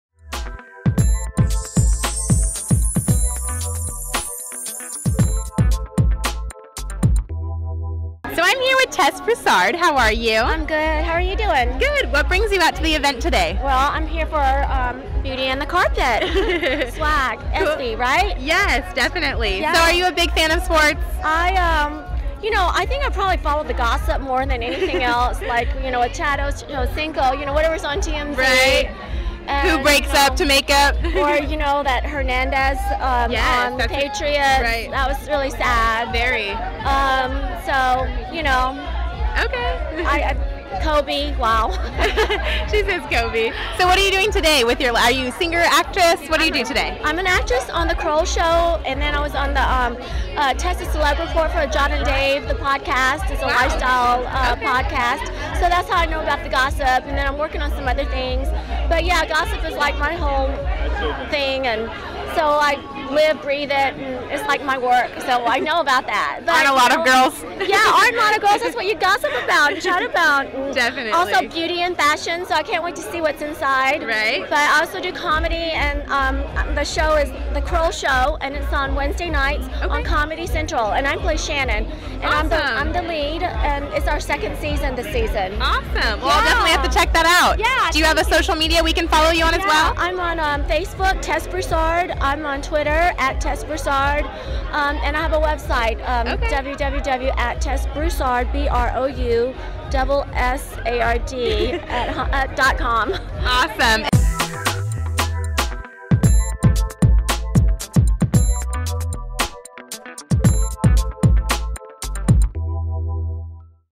Actress Tess Broussard Chats with BeautyNCarpet
The lovely Tess Broussard chats with SPTBtv about upcoming projects and her interest in celebrity and entertainment news.
2013 Espy's Beauty and the Beast Swag Lounge event.

SPTBtv
SeePastTheBeauty Entertainment

"Beauty is an experience, not a label"